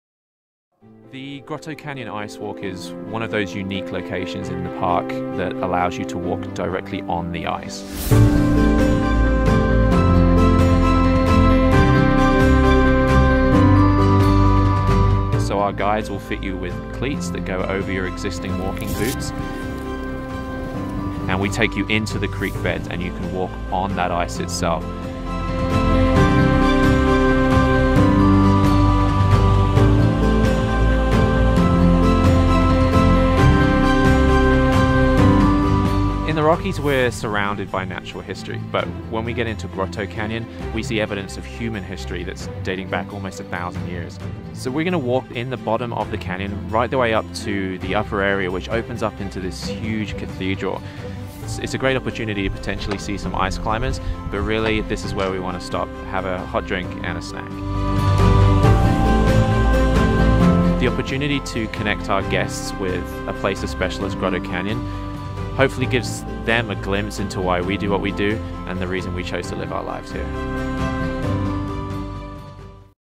Grotto Canyon Icewalk Tour, Banff, Breakaway Experiences
Hike on a frozen creek bed, through a narrow canyon, past unusual rock formations and spectacular icefalls on the Grotto Canyon Icewalk, Banff, Alberta

A short drive just outside Banff National Park, inside the Grotto Canyon, you’ll walk on a frozen creek into Grotto Canyon, past unusual rock formations, ancient Native rock art, and icefalls. The history of the rock art and the geology of the mountains will be at your fingertips as your guide explains their mysteries.

You might see ice climbers scaling the frozen waterfall, but the highlight of the Grotto Canyon Icewalk is the ancient native pictographs, easily missed by the general public. There is a theory that one of the symbols could be a Kokapelli, a flute player painted in ochre, used only by the Hopi people, some 500-1300 years ago. The symbols have many possible meanings and origins and you will learn more about First Nation history in the Bow Valley.